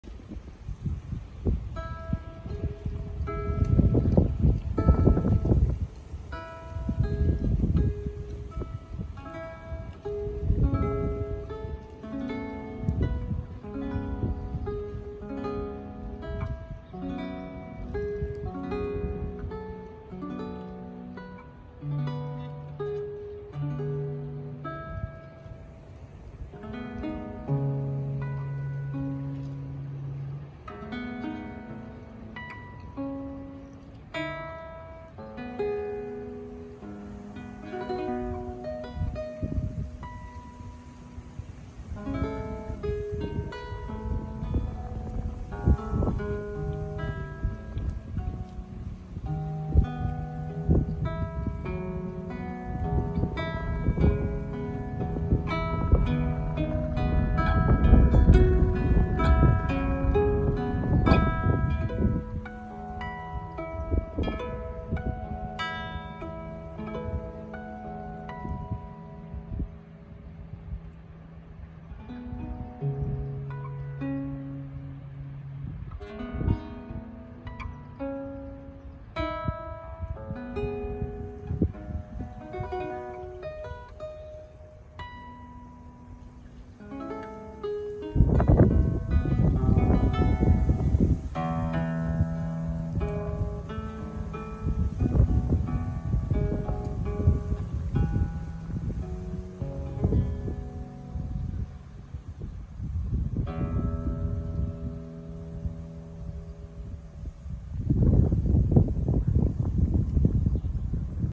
Romantic guitar Jan F. Veselý
Lukáš Sommer playing on my guitar